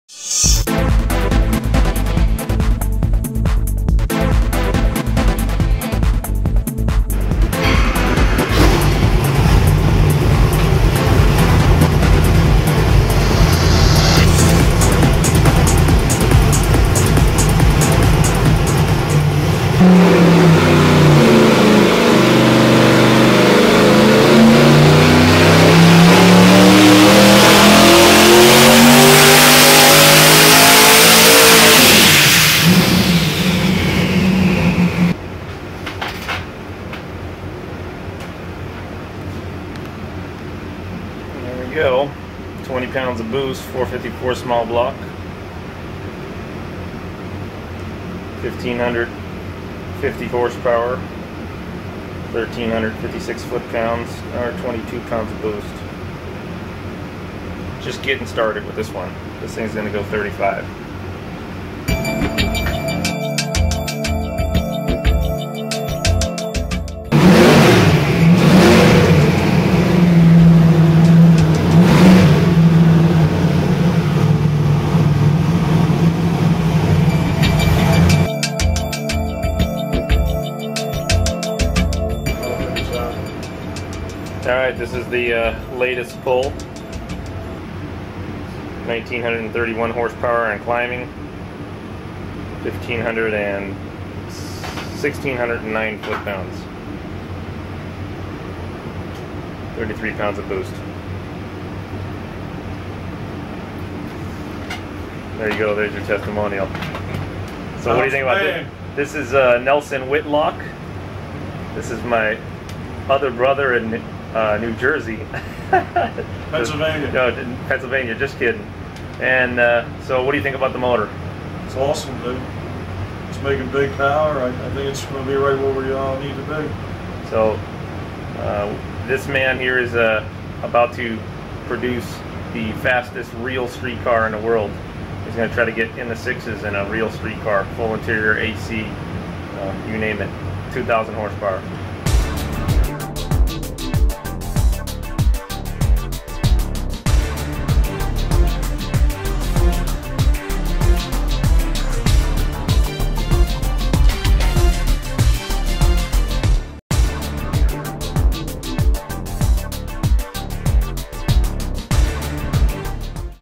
Nelson Racing Engines 2300 HP 454 TT SBC. Record breaking!
NRE produces 2300 HP SBC at 38 Lbs. boost.  Watch this video!  Download it for your computer or IPOD